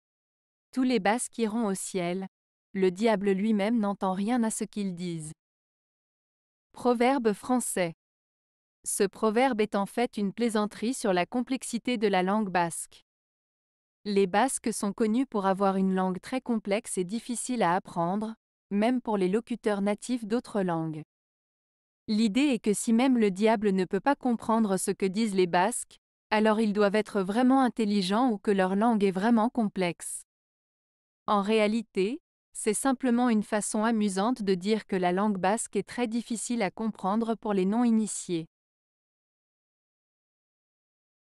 Tous les basques iront au ciel - Le diable lui-même n'entend rien à ce qu'ils disent.
Ce proverbe est en fait une plaisanterie sur la complexité de la langue basque. Les Basques sont connus pour avoir une langue très complexe et difficile à apprendre, même pour les locuteurs natifs d'autres langues. L'idée est que si même le diable ne peut pas comprendre ce que disent les Basques, alors ils doivent être vraiment intelligents ou que leur langue est vraiment complexe ! En réalité, c'est simplement une façon amusante de dire que la langue basque est très difficile à comprendre pour les non-initiés.